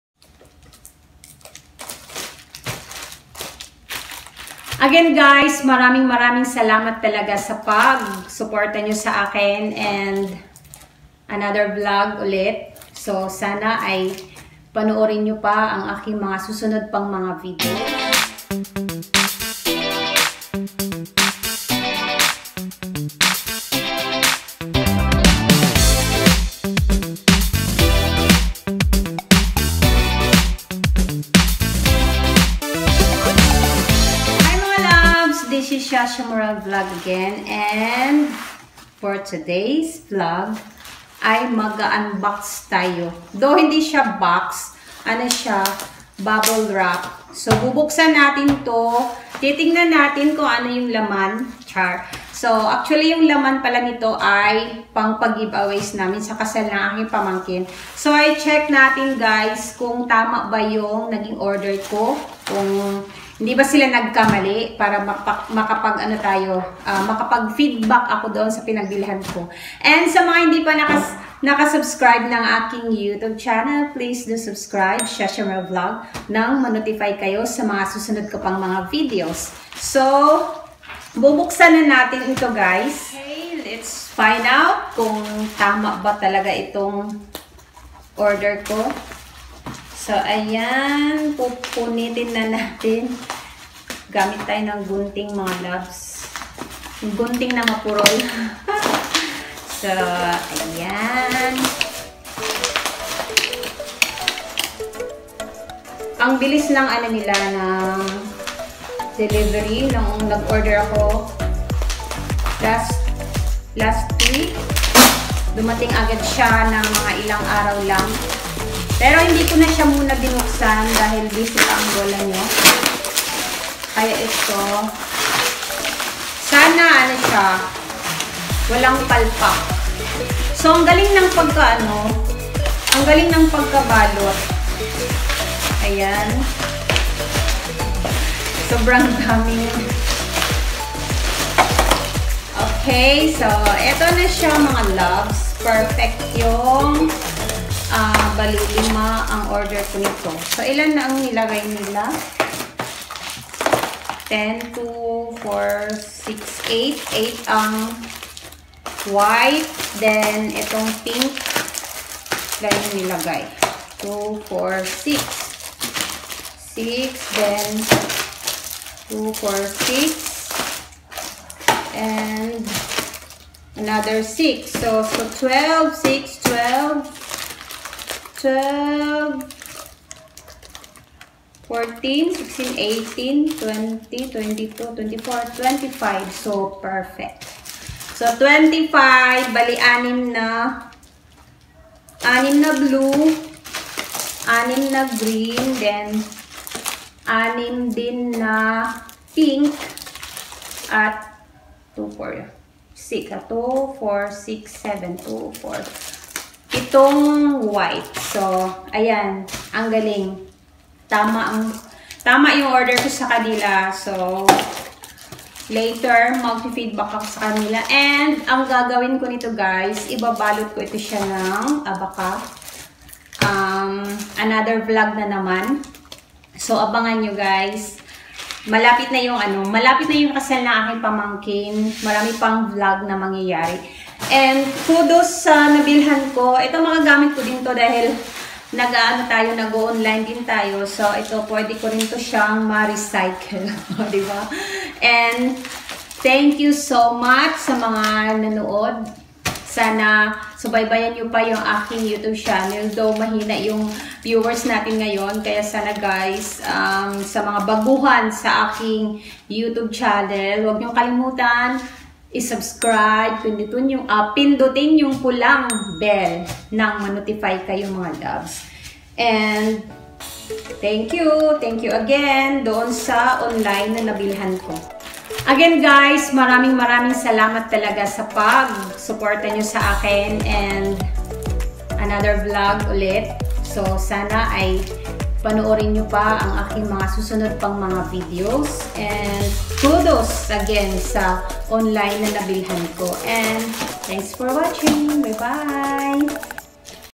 Unboxing | Case Disposable Mask
Unboxing my parcel that I bought from Shopee and it was perfect i got the right items so kudos to the owner the online definitely will order again soon.